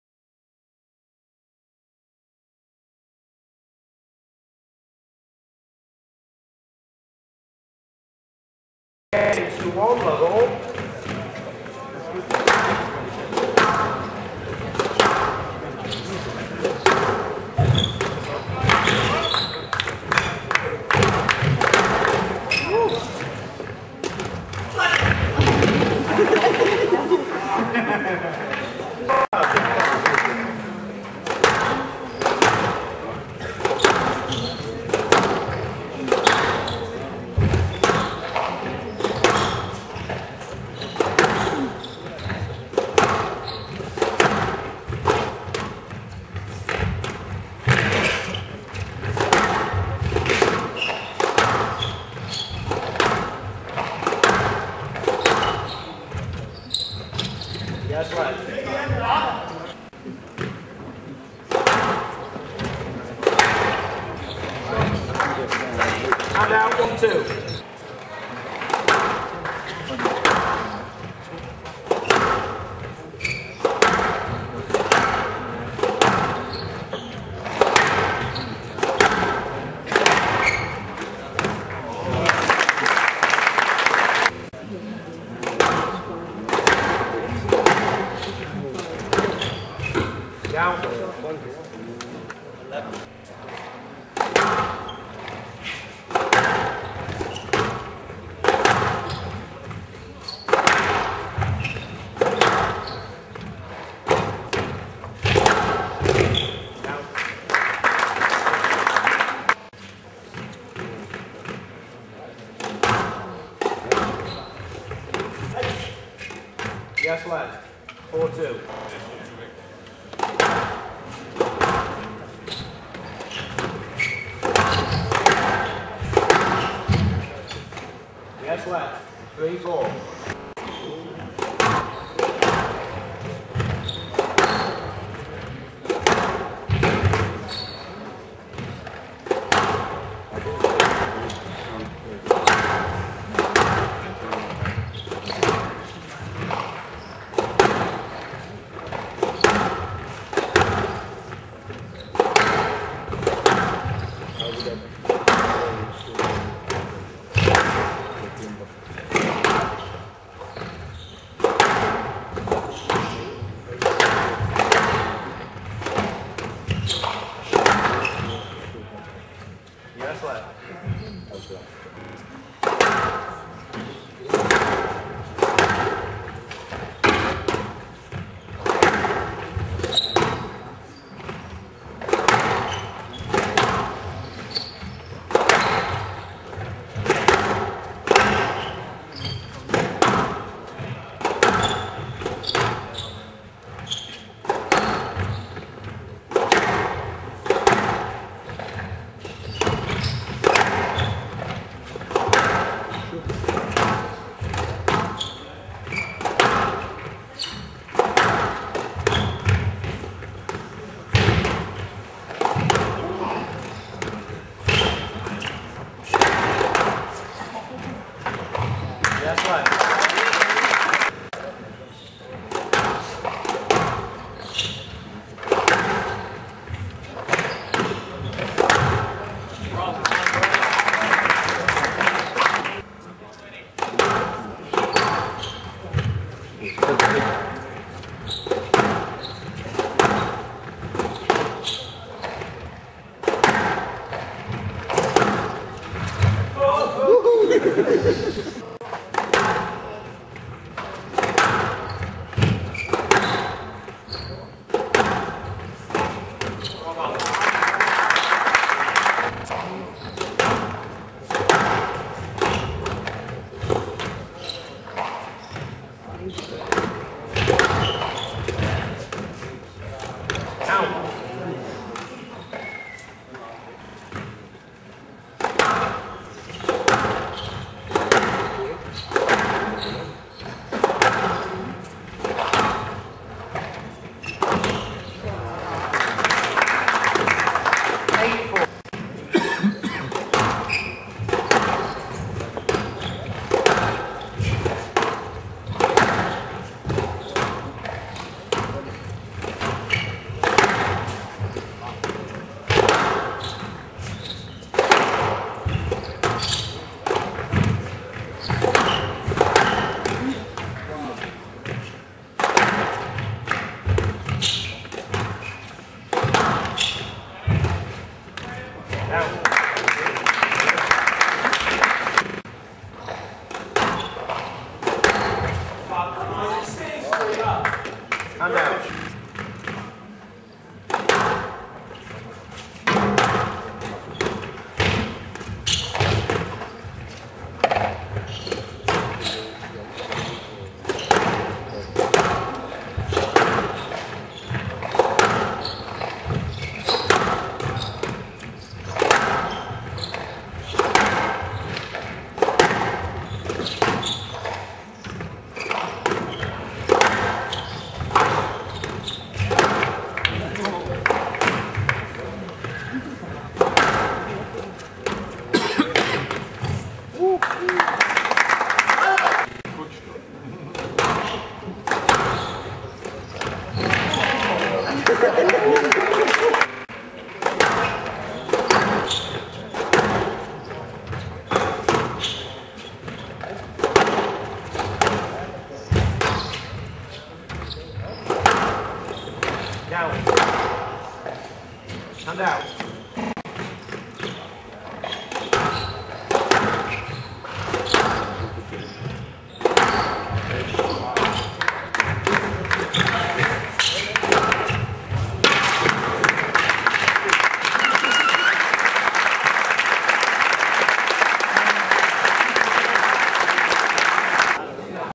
AmrShabana v JonathonPower Game4
EGYPT-V-CANADA,
WORLD MEN'S SQUASH TEAM CHAMPIONSHIP,ODENSE
EN GOD OPLEVELSE